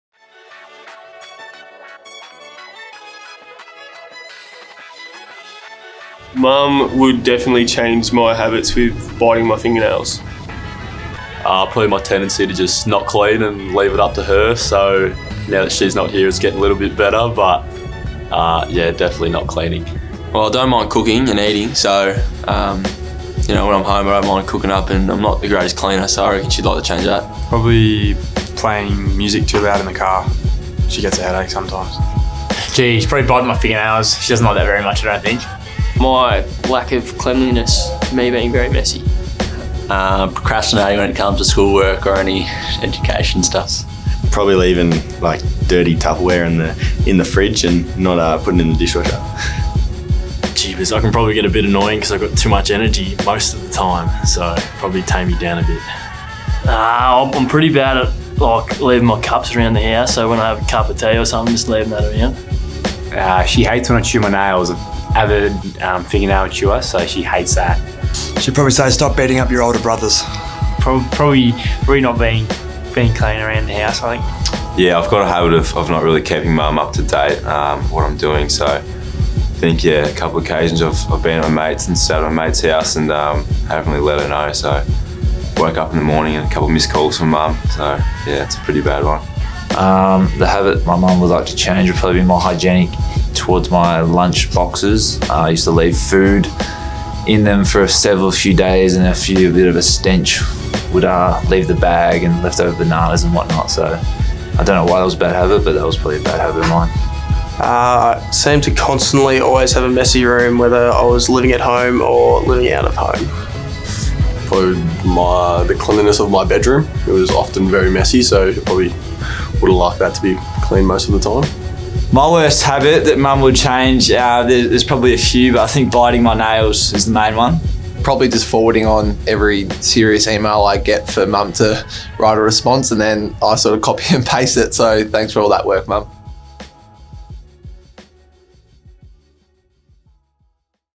A GIANT Mothers Day - Your Worst Habit
We ask the GIANTS what is the worst Habit that your Mother would like to change in you.